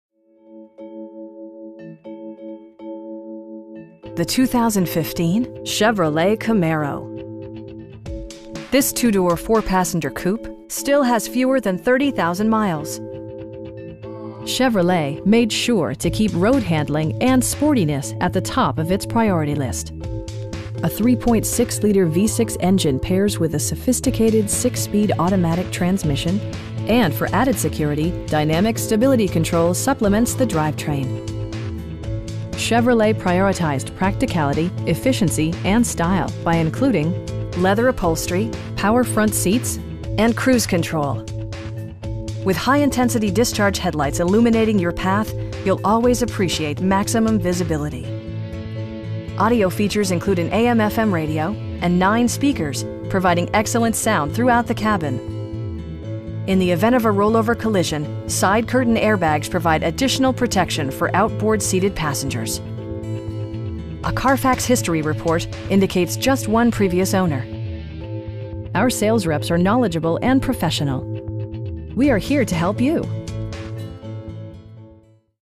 2015 Chevrolet Camaro LT w/2LT in Hannibal, MO 63401-3614
Poage Chrysler Dodge Jeep Ram FIAT
900 Clinic Rd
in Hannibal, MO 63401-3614

The 2015 Chevrolet Camaro. This 2 door, 4 passenger coupe still has fewer than 30,000 miles! Chevrolet made sure to keep road-handling and sportiness at the top of it's priority list. A 3.6 liter V-6 engine pairs with a sophisticated 6 speed automatic transmission, and for added security, dynamic Stability Control supplements the drivetrain. Chevrolet prioritized practicality, efficiency, and style by including: leather upholstery, 1-touch window functionality, power front seats, automatic dimming door mirrors, heated seats, power windows, and cruise control. The unique heads-up display projects vehicle information onto the windshield, including speed, gear selection and engine speed. Drivers benefit by not having to take their eyes off the road. With high intensity discharge headlights illuminating your path, you'll always appreciate maximum visibility. Audio features include an AM/FM radio, steering wheel mounted audio controls, and 9 speakers, providing excellent sound throughout the cabin. In the event of a rollover collision, side curtain airbags provide additional protection for outboard seated passengers. A Carfax history report indicates just one previous owner. Our sales reps are knowledgeable and professional. We'd be happy to answer any questions that you may have. We are here to help you.